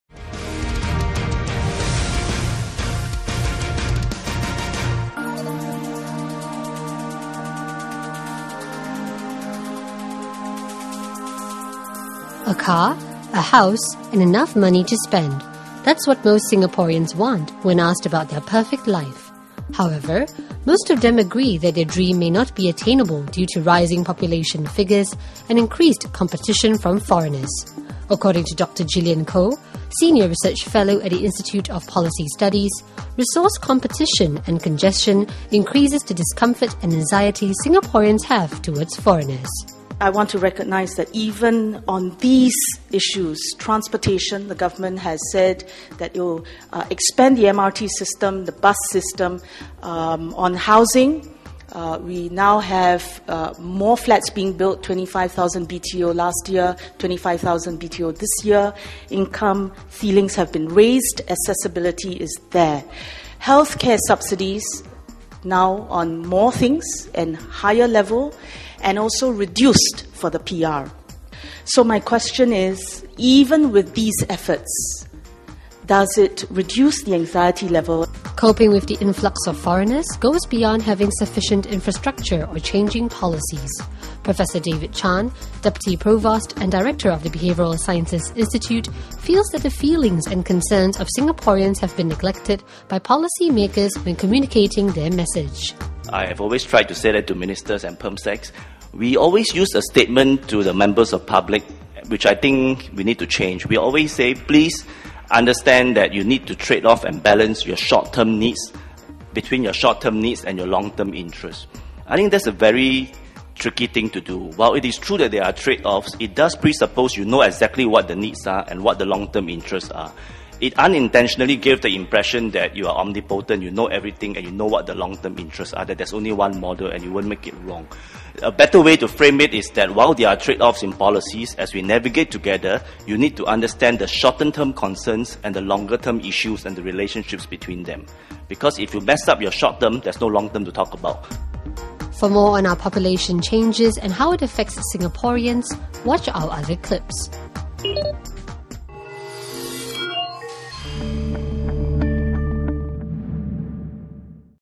How to reduce intolerance towards foreigners
Instead of asking Singaporeans to give up short term needs for long-term benefits when it comes to immigration policies, Professor David Chan from the Behavioural Sciences Institute thinks policy makers need to change the way they convey messages to the public.